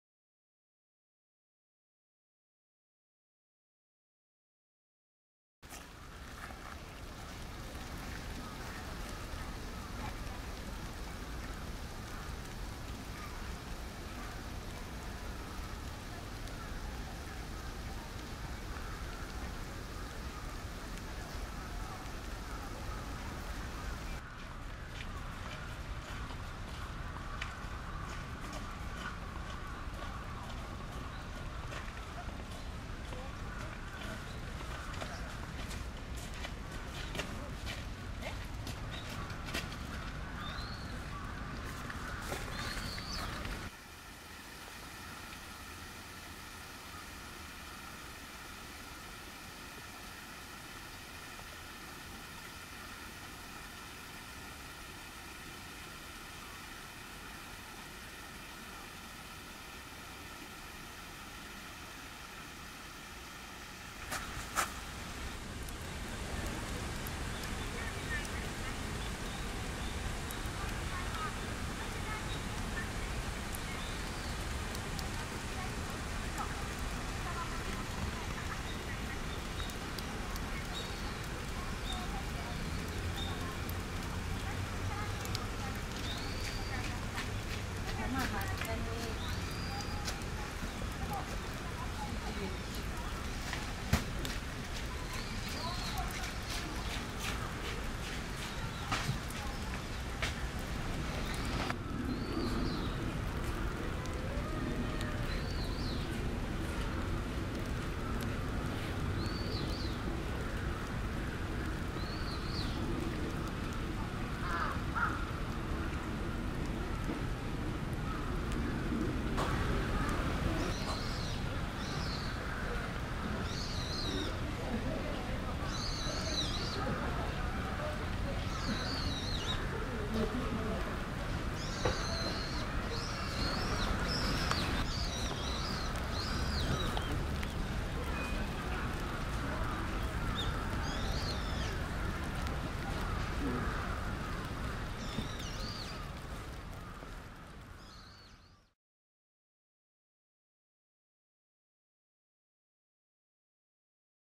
Snowing in Kenroku-en (兼六園 ) Garden - Kanazawa (金沢市), Japan 2012
It is considered to be one of the most beautiful gardens in Japan (may be The most beautiful). These are just a few glimpses of it during heavy snowfall.